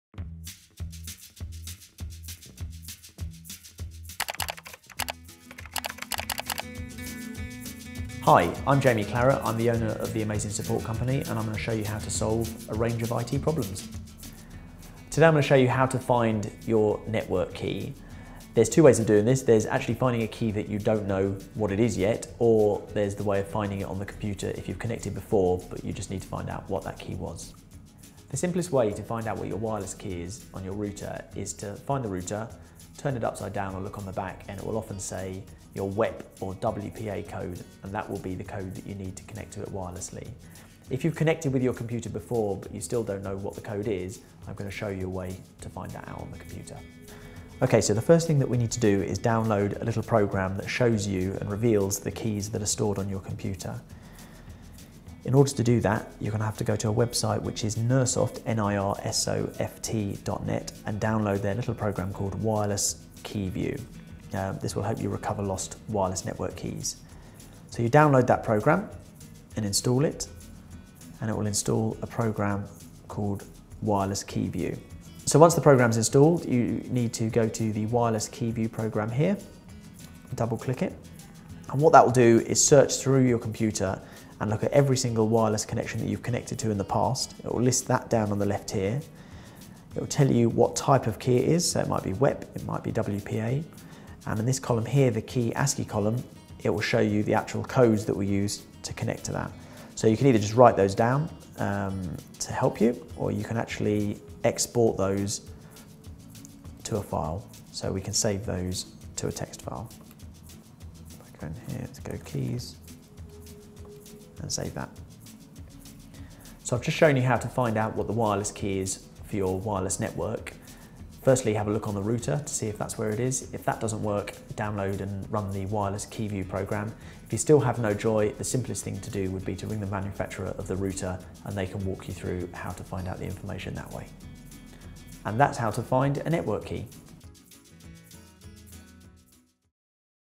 How To Find The Network Key On Your Computer
This guide shows you How To Find Network Key